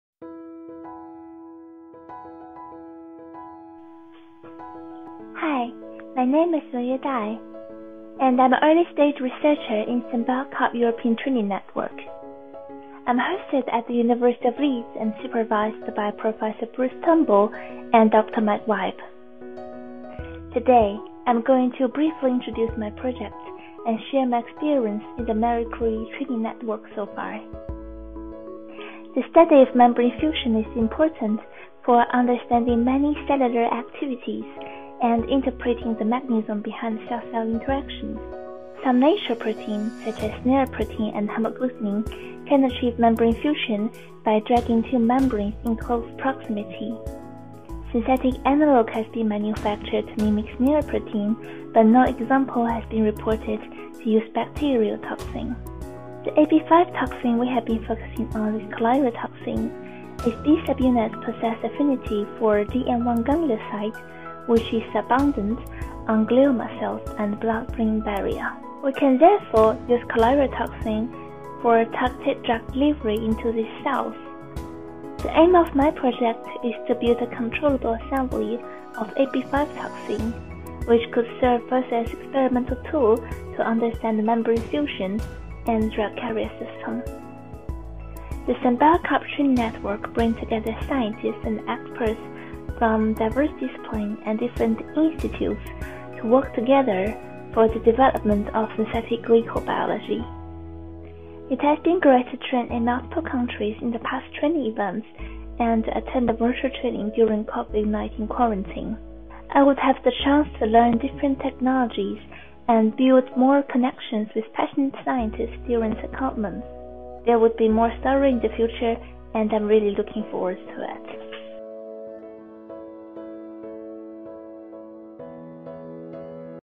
Diary of a Scientific Researcher : Wenyue Dai (ESR1) Chapter One
This is an introductory video from Wenyue (Eva) Dai who is an early stage researcher (ESR) in the synBIOcarb Innovative Training Network working on synthetic glycobiology.